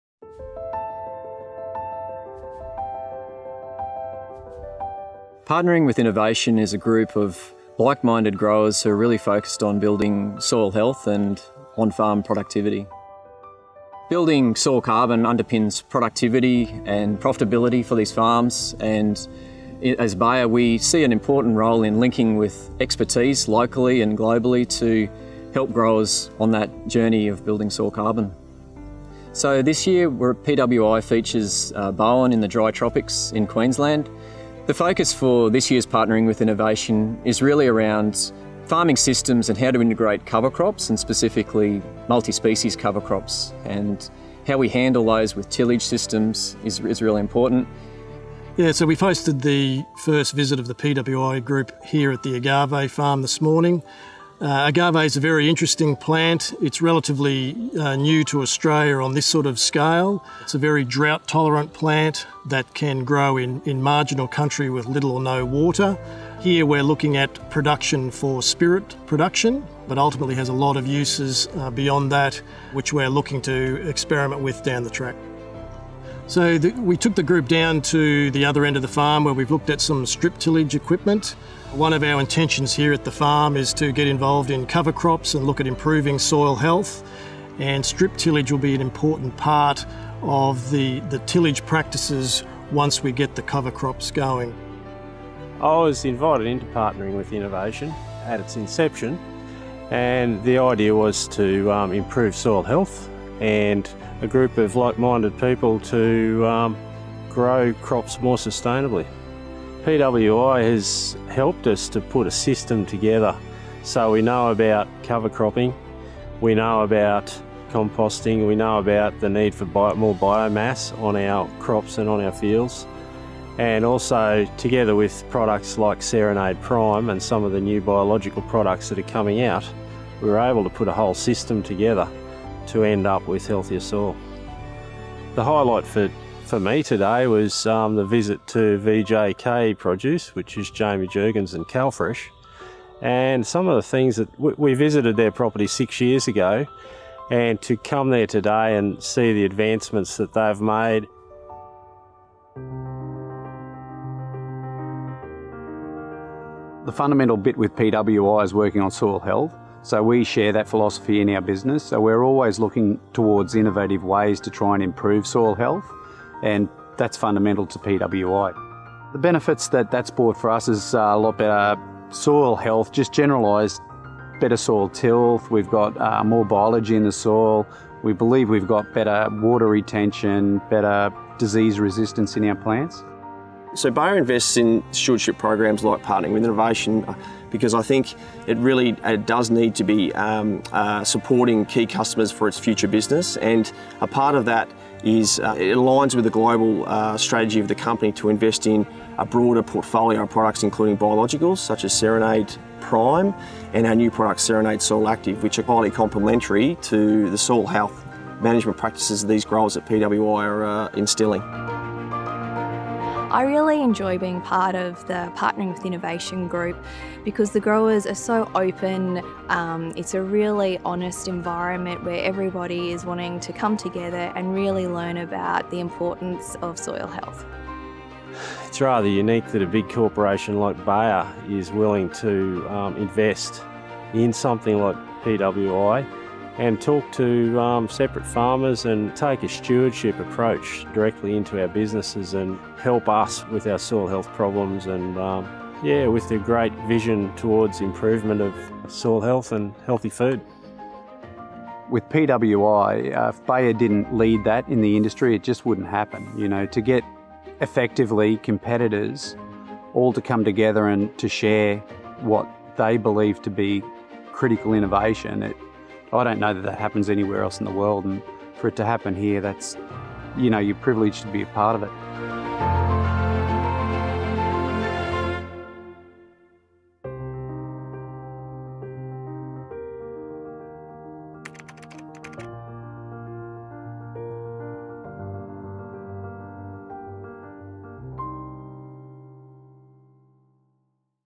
Regenerative farming practice knowledge sharing to build soil health
Partnering with Innovation (PWI) brings together a diverse group of Australian farming leaders at the forefront of regenerative agricultural practices in their production systems. Hosted by Bayer, in 2021 PWI was held in Bowen in the Queensland dry tropics. The continued focus by the group on soil health and on-farm productivity was strengthened with firsthand insights and experience in the region with multispecies cover cropping combined with the necessary tillage systems to support and enhance the farming system.